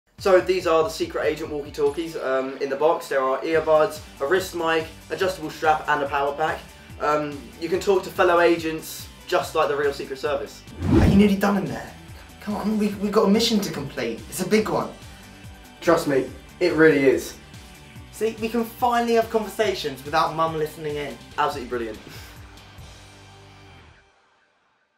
SpyX Secret Agent Walkie Talkies - Demo Video
Product Demo of the SpyX Secret Agent Walkie Talkies, from Trends UK Ltd.
Communicate in secret, just like real secret service agents.
Use the Secret Agent Walkie Talkie to talk to your fellow agents, just like real secret service agents. Beautifully designed for ultimate stealth mode. Talk into the mic on your adjustable wrist strap and the receiver with belt clip will transmit the messages to your fellow agents ear buds. Voice activated for comfortable hands free spying . Features 2 x wrist walkie talkies, 16 channels, channel scan, LCD display.